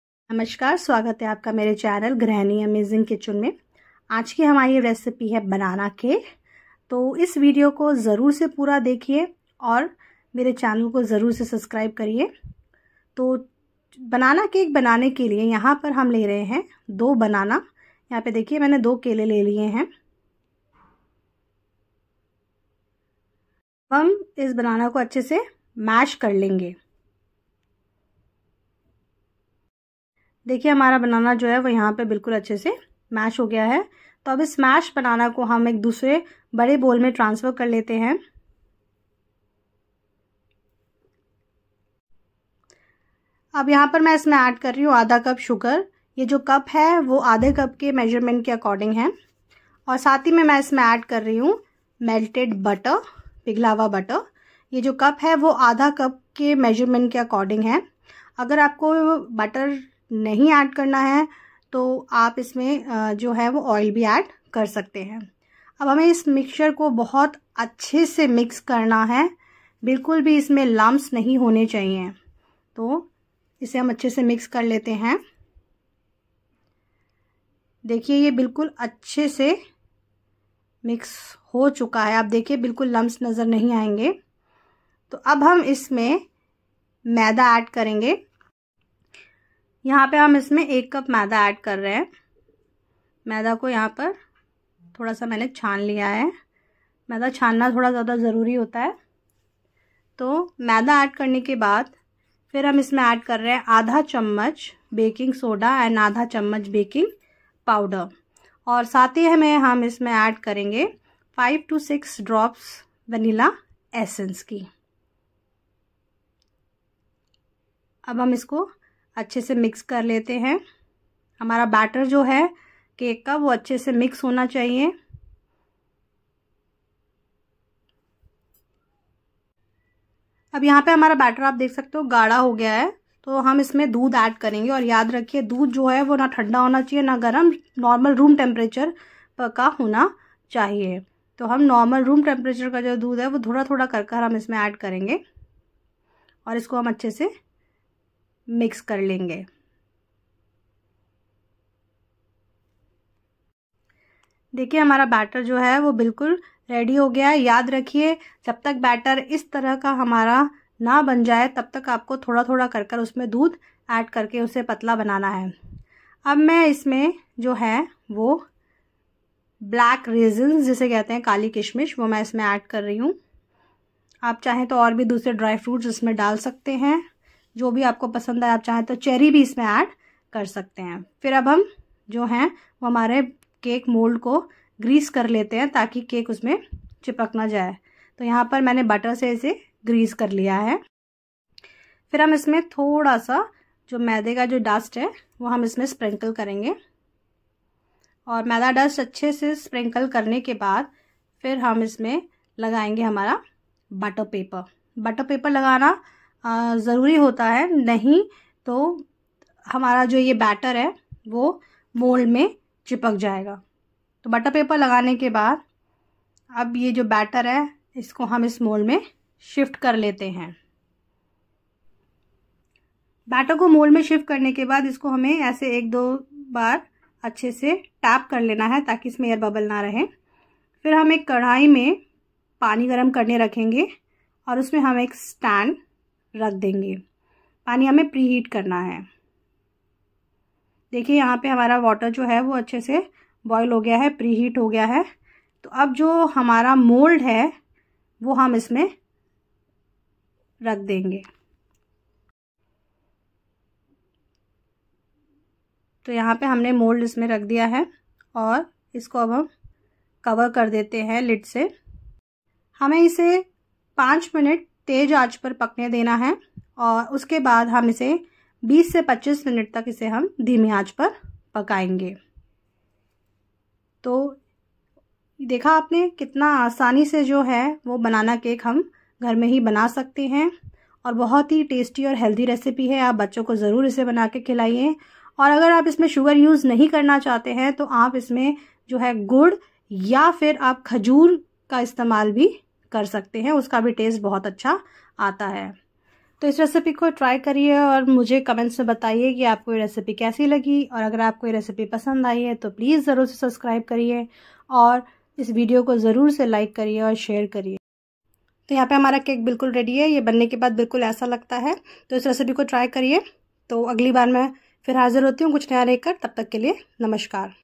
Soft And Spongy Banana Cake | Eggless Cake Without Oven| Homemade Recipe | Healthy And Tasty Recipe
Ingredients :
Banana- 2
Powdered Sugar- 1/2 cup
Melted butter/ oil - 1/2 Cup
Black Raisins ( kismiss) or dry fruits or tutty Fruity

From:
Grahaniamazingkitchen